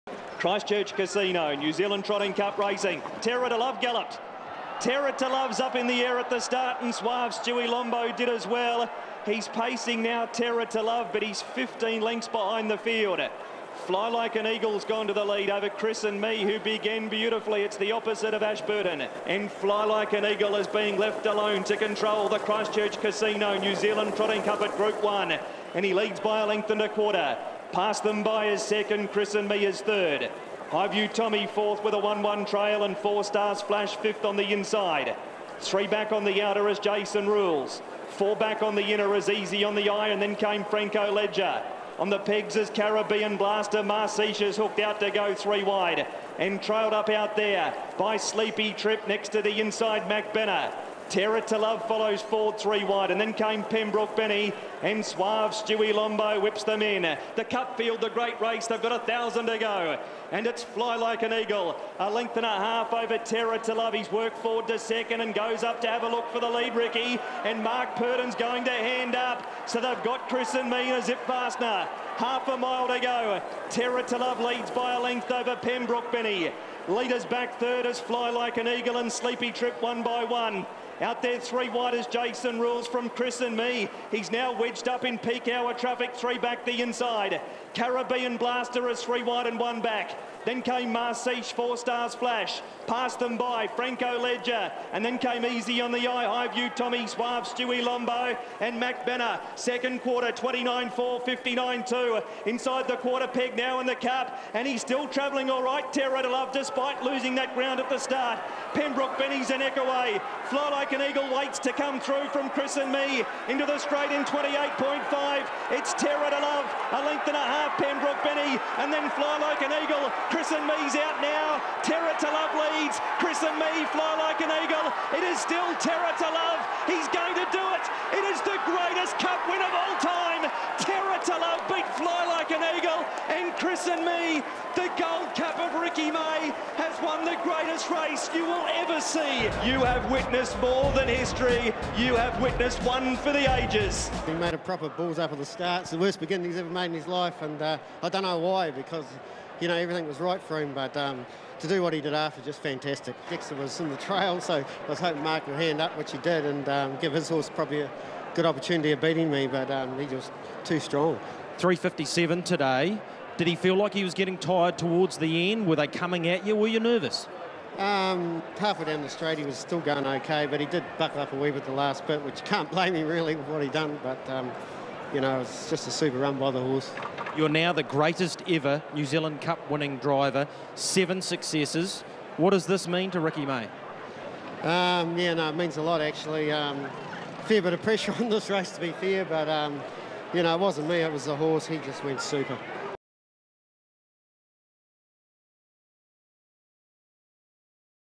History !
HRTV's 'Most Pop' clip of 2013!.Rick May 7!'Terror To Love' 3!.The biggest reign since 1960 on NZ Cup Day at Addington!